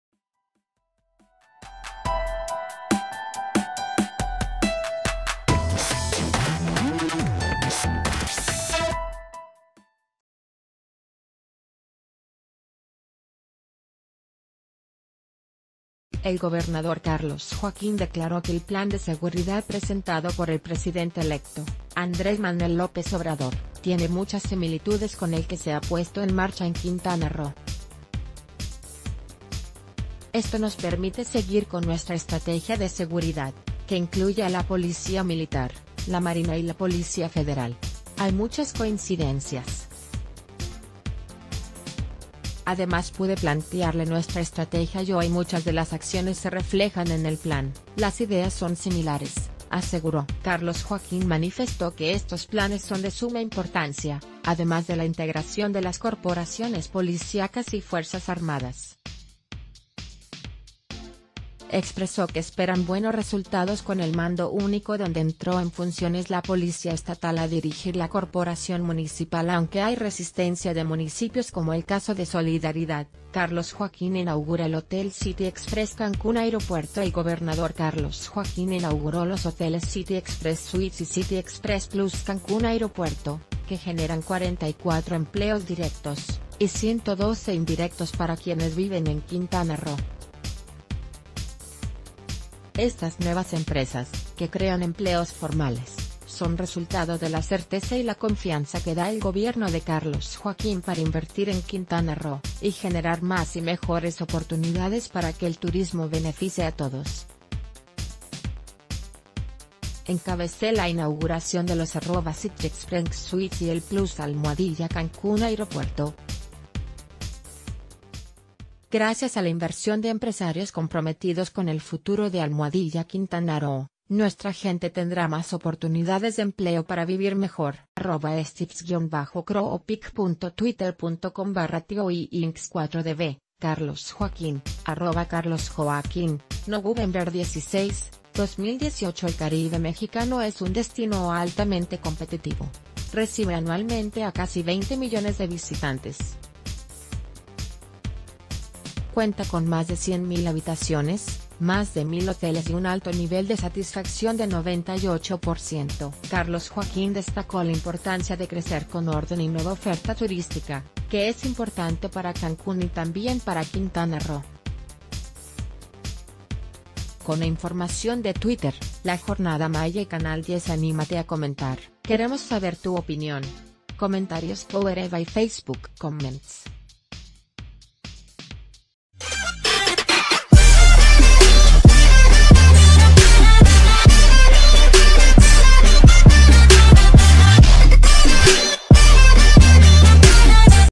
Carlos Joaquín respalda plan de seguridad que impulsa AMLO
El gobernador Carlos Joaquín declaró que el plan de seguridad presentado por el presidente electo, Andrés Manuel López Obrador, tiene muchas similitudes con el que se ha puesto en marcha en Quintana Roo. ‘Esto nos permite seguir con nuestra estrategia de seguridad, que incluye a la Policía Militar, ...